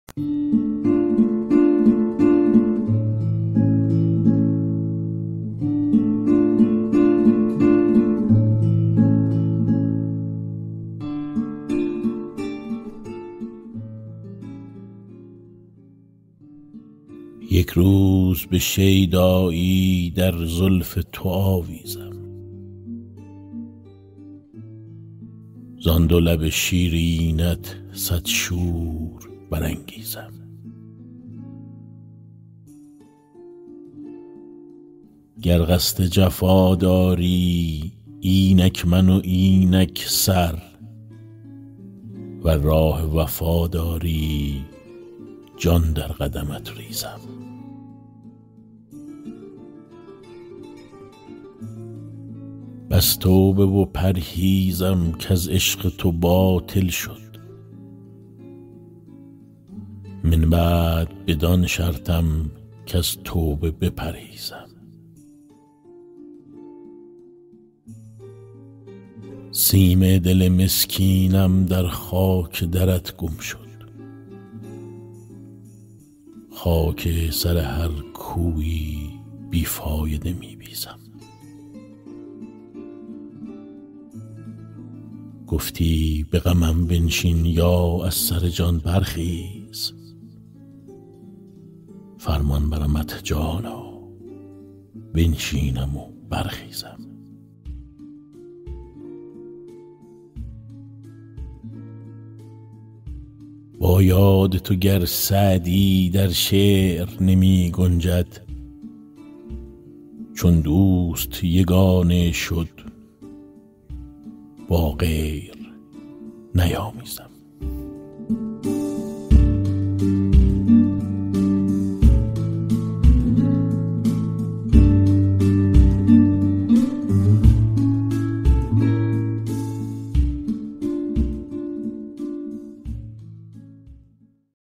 یک روز به شیدایی در زلف تو آویزم-غزل ۴۰۱ سعدی با صدای بهزاد جعفرپور در شعر خوانی
یک روز به شیدایی در زلف تو آویزم
زان دو لب شیرینت صد شور برانگیزم

گر قصد جفا داری اینک من و اینک سر
ور راه وفا داری جان در قدمت ریزم

بس توبه و پرهیزم کز عشق تو باطل شد
من بعد بدان شرطم کز توبه بپرهیزم

سیم دل مسکینم در خاک درت گم شد
خاک سر هر کویی بی فایده می‌بیزم

گفتی به غمم بنشین یا از سر جان برخیز
فرمان برمت جانا بنشینم و برخیزم

با یاد تو گر سعدی در شعر نمی‌گنجد
چون دوست یگانه شد با غیر نیامیزم

غزل ۴۰۱ سعدی
اجرا : بهزاد جعفرپور
موسیقی